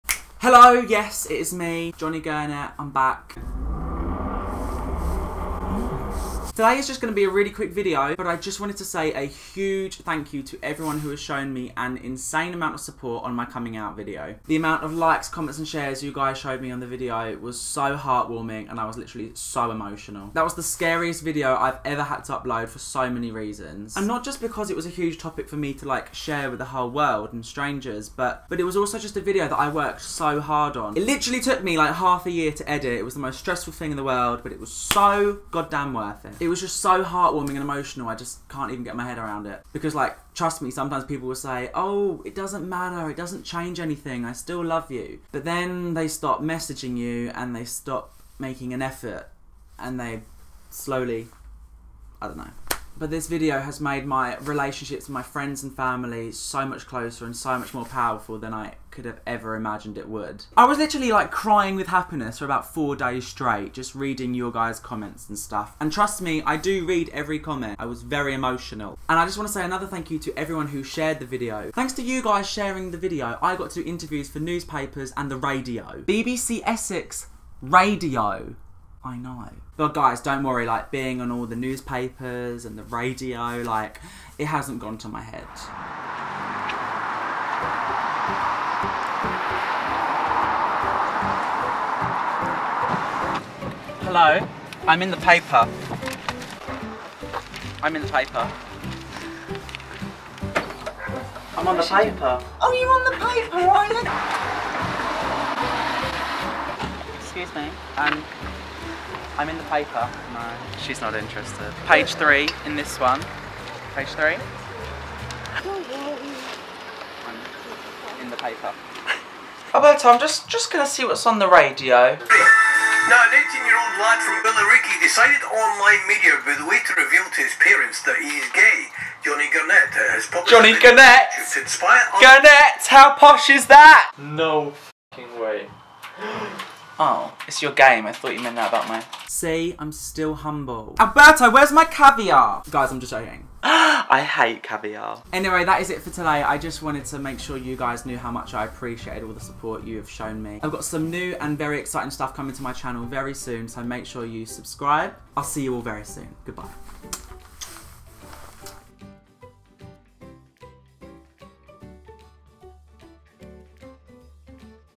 People's Reaction to me Coming Out Gay.
About peoples reactions to me being gay...

Just a short fun one.

EXCITING STUFF IS COMING.